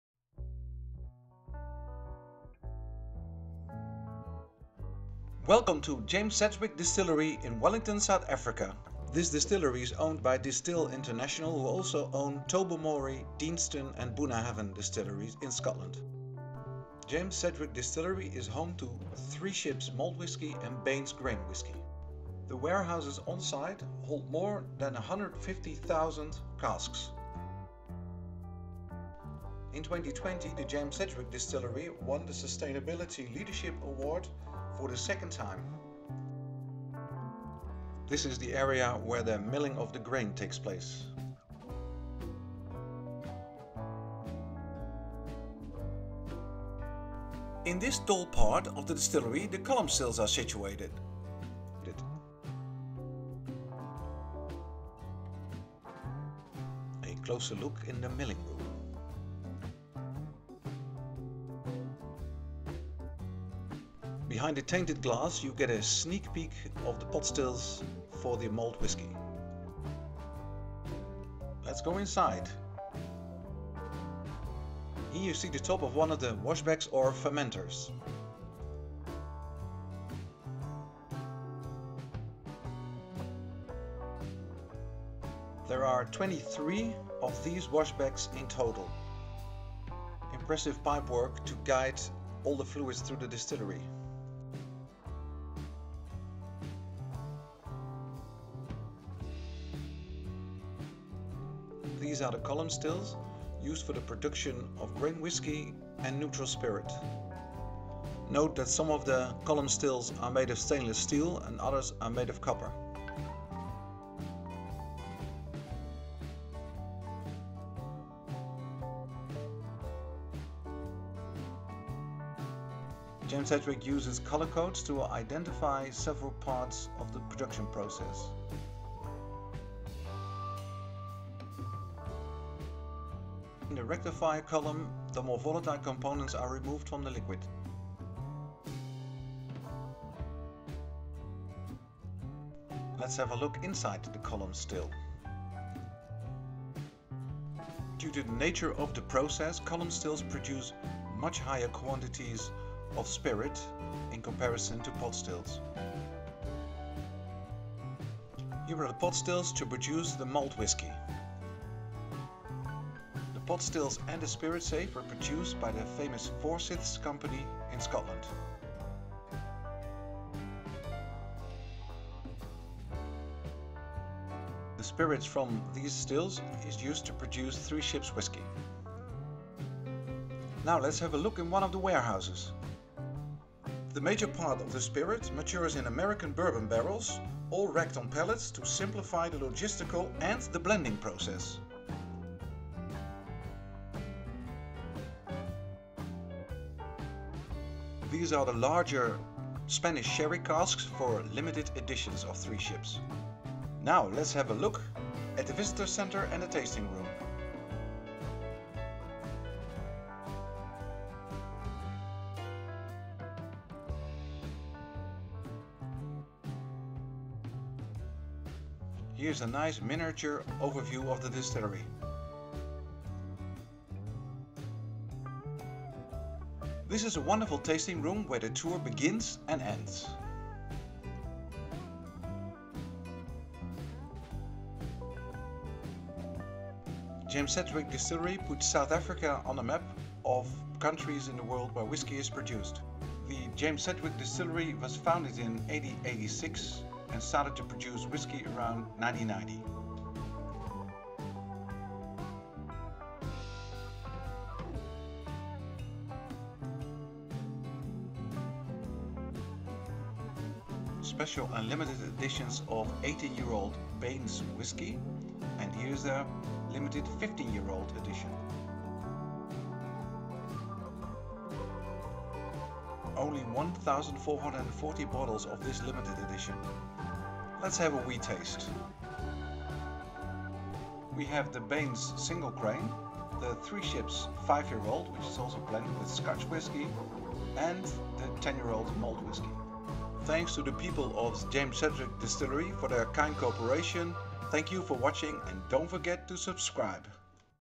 The James Sedgwick Distillery, Wellington, South Africa
Impression of the James Sedgwick Distillery in Wellington in the Western Cape of South Africa.
This distillery combines both grain and malt whisky production.
Main products are Three Ships malt whisky and Bain's grain whisky.
The distillery belongs to Distell International, which is also owner of Bunnababhain, Tobermory and Deanston distilleries in Scotland.
Thanks to Nathalie for logistical support and advice.
Thanks to the people of the James Sedgwick Distillery for their kind support.